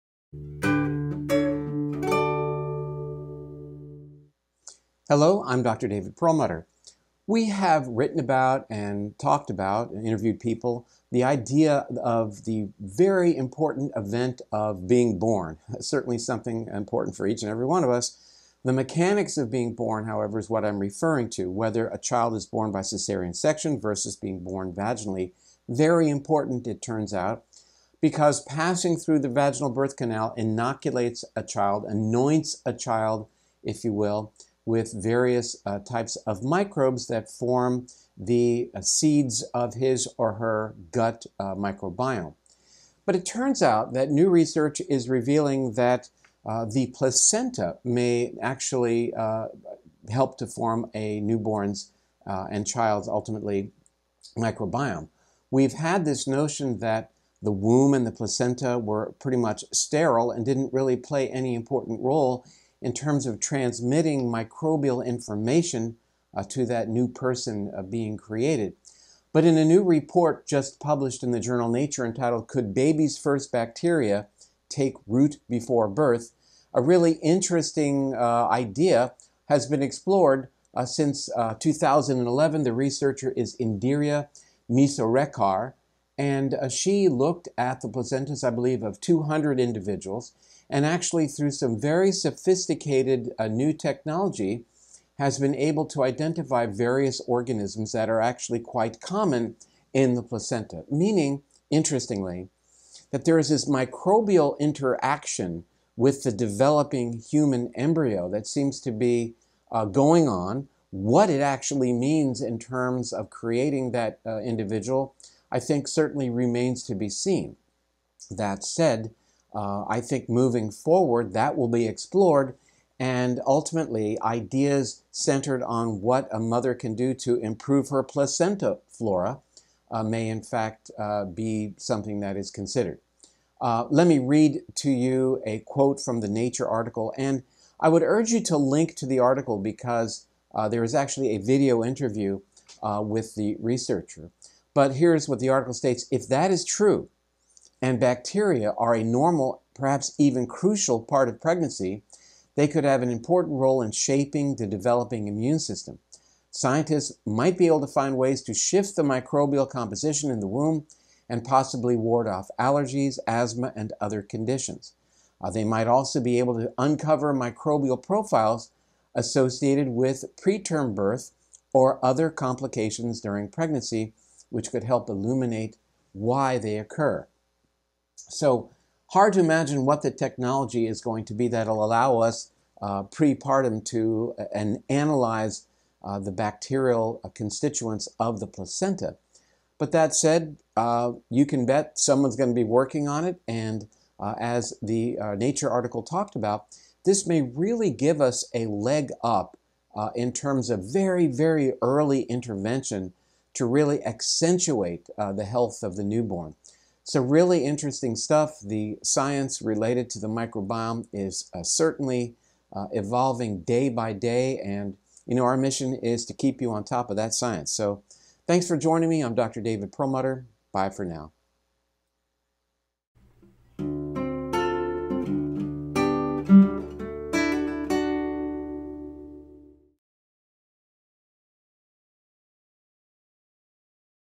Baby's First Bacteria: When Does the Microbiome Begin?
We often talk about the importance of method of birth on a newborn's health, not only in the short-term, but in the long-term as well. Research has found, again and again, that vaginal birth, and the experience of going through the birth canal, exposes a child to various microbes that form the basis for their own gut microbiome.

I'm not here to dive into that research today though (but you can learn more Excitingly, new research is finding that the placenta may actually play a significant role in microbiome development. This would change everything we know about the placenta's purpose! Let's take a deeper look.